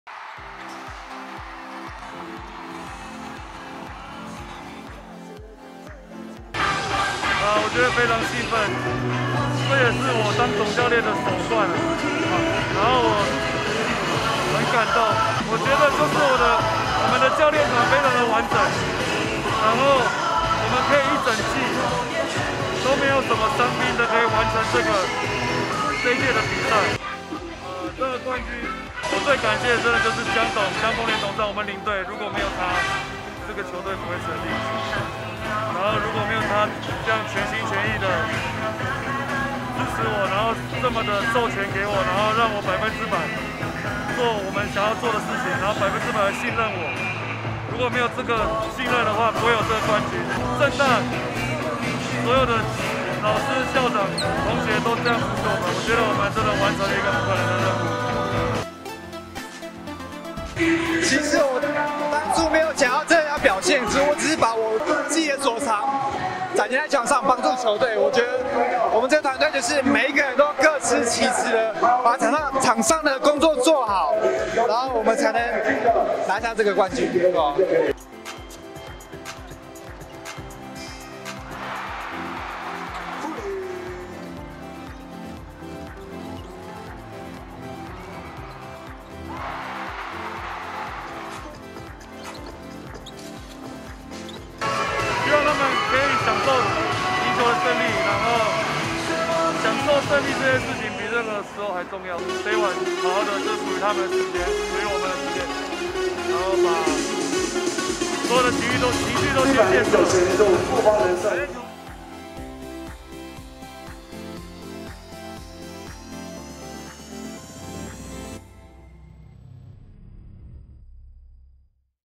《富邦人壽UBA》首度攻蛋就奪冠 政大雄鷹興奮寫隊史
首度攻進臺北小巨蛋的 政大雄鷹，接連擊敗衛冕軍健行科大與力拚男女雙冠的世新大學，第一次進到小巨蛋，就風光搶下隊史第一座冠軍，全場政大加油團興奮慶賀！

「這是我執教以來的第一座冠軍，感謝姜豐年董事長的全力支持，才讓我們完成我們想做的事！」 陳子威 賽後感性表示，這一冠有太多人的努力，包括學校、老師與學生的相挺，回想奮鬥的這一切，他說：「感謝我的家人無私的支持，讓我能全心全力去完成目標！」

全場攻下25分的 張鎮衙，獲選冠軍賽MVP，他感謝隊友一起努力，才讓他獲得這樣的榮耀，他只是不斷在球場上打出自己最擅長的方式，去幫助球隊取勝，他說：「全隊都是MVP！」